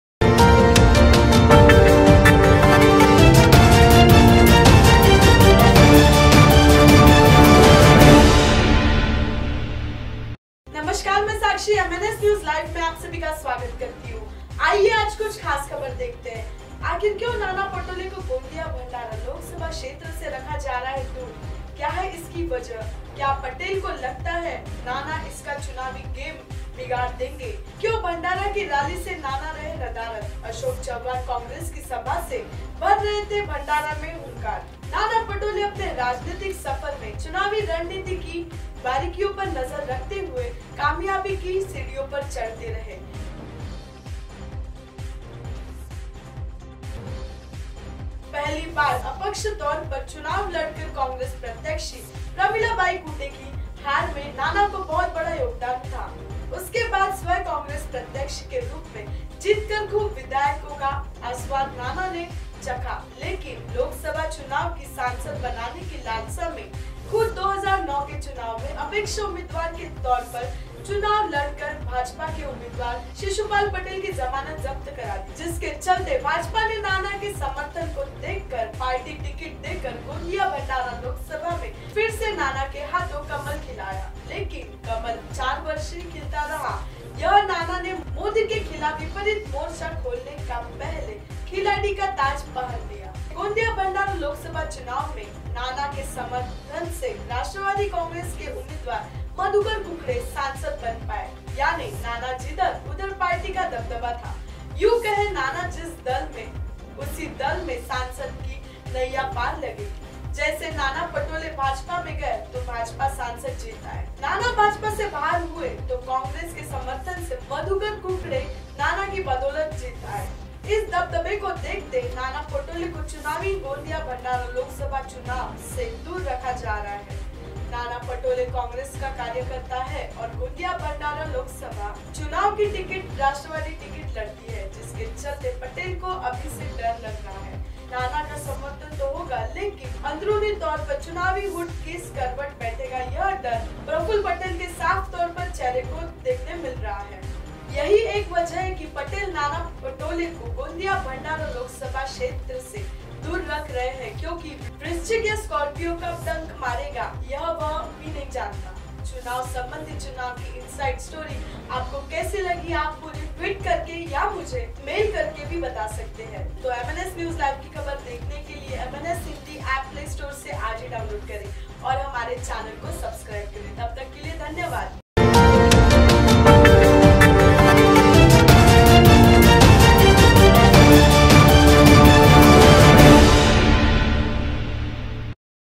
MNS NEWS LIVE IN HINDI पटेल आखिर क्यू डरते है नाना पटोले से? गोंदिया -भंडारा
MNSNEWSHINDILIVE गोंदिया -भंडारा लोकसभा चुनाव क्षेत्र से क्यू रखा जारहा ही दूर नाना पटोले को ,पटेल आखिर क्यू डरते है नाना पटोले से?